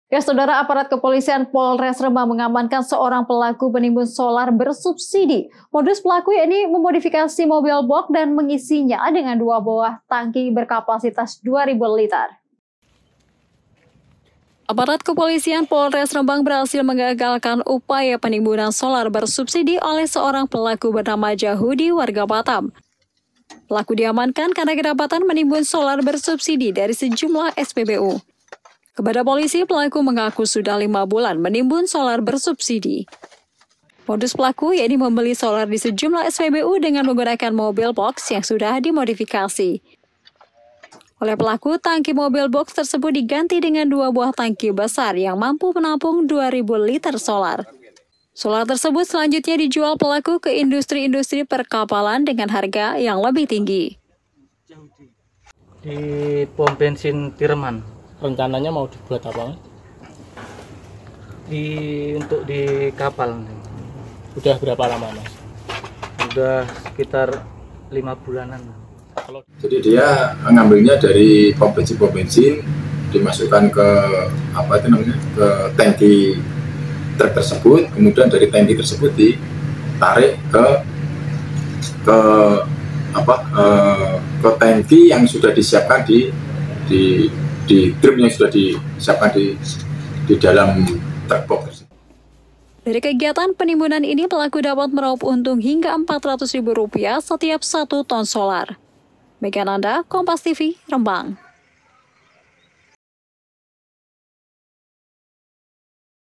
Polisi Tangkap Penimbun Solar Subsidi
PEKALONGAN, KOMPAS.TV - Aparat Kepolisian Polres Rembang berhasil menggagalkan upaya penimbunan solar bersubsidi oleh seorang pelaku bernama Jahudi warga Batam. Pelaku diamankan karena kedapatan menimbun solar subsidi dari sejumlah SPBU. Kepada Polisi, pelaku mengaku sudah lima bulan menimbun solar bersubsidi .

Modus pelaku yakni membeli solar di sejumlah SPBU dengan menggunakan mobil boks yang sudah dimodifikasi. Oleh pelaku tangki mobil boks tersebut diganti dengan dua buah tangki besar yang mampu  menampung 2 ribu liter solar. Solar tersebut selanjutnya dijual pelaku ke industri-industri perkapalan dengan harga yang lebih tinggi.

Dari kegiatan penimbunan ini, pelaku dapat meraup untung hingga 400 ribu rupiah setiap satu ton solar.